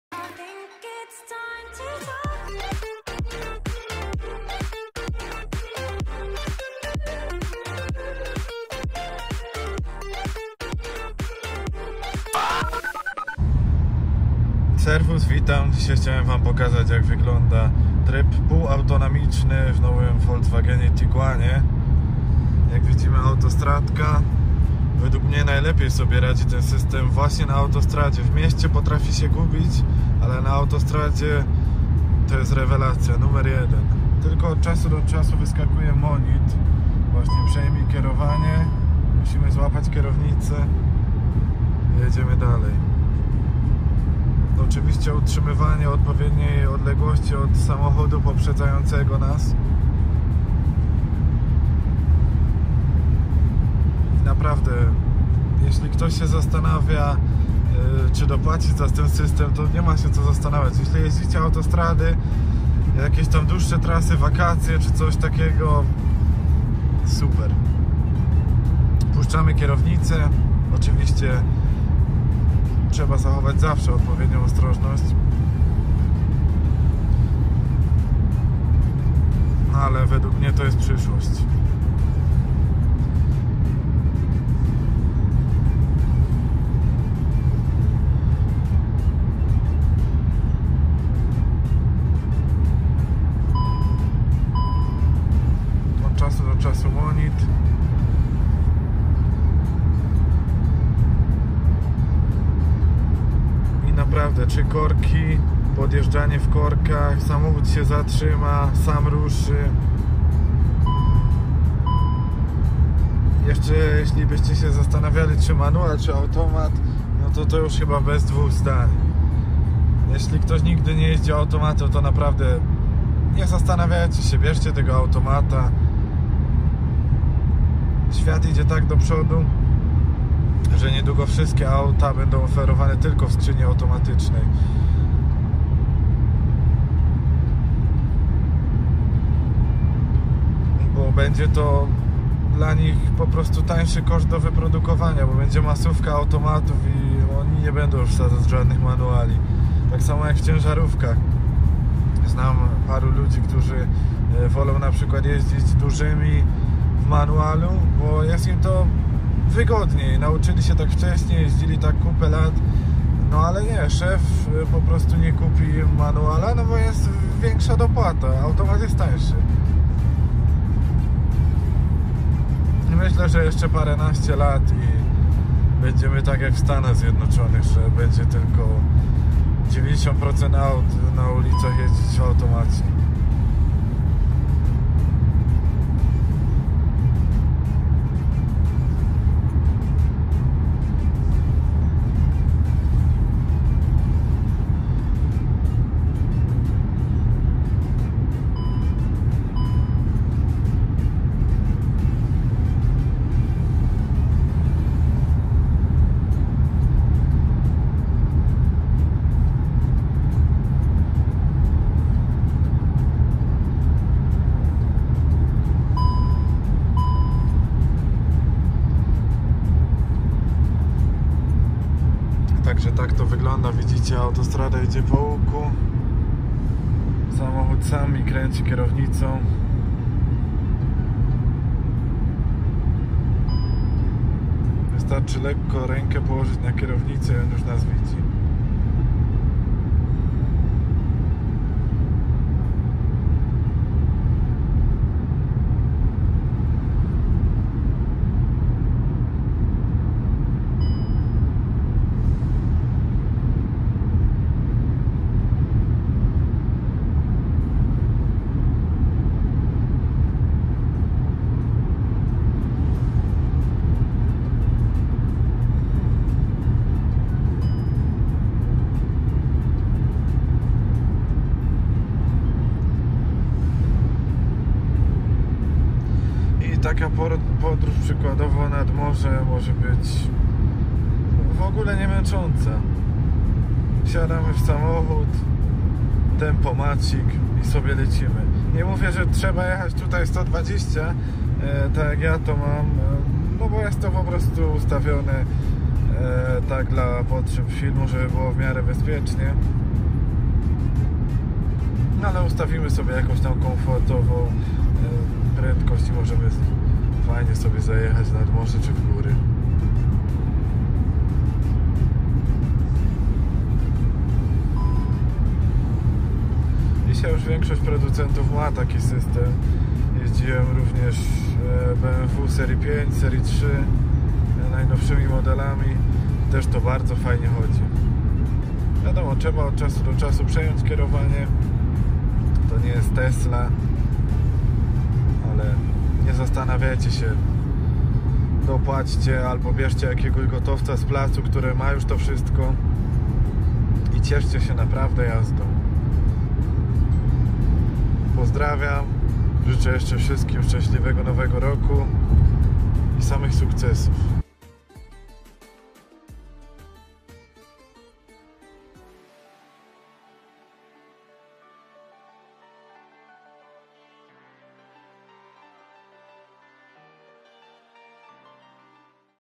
AUTONOMICZNA JAZDA VW TIGUAN 2021 - CZY TO MOŻLIWE? driveassist vw tiguan 2021
WESPRZYJ KANAŁ - TO DZIĘKI TOBIE BĘDĘ MÓGŁ NAGRYWAĆ NOWE FILMY !
W NASTĘPNYM FILMIE PODZIĘKOWANIE SPECJALNIE DLA CIEBIE!